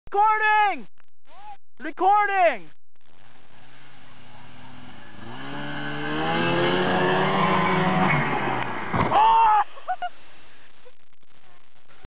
Pipat from thailand getting owned by a snow mobile
This is pipat jumping a snow mobile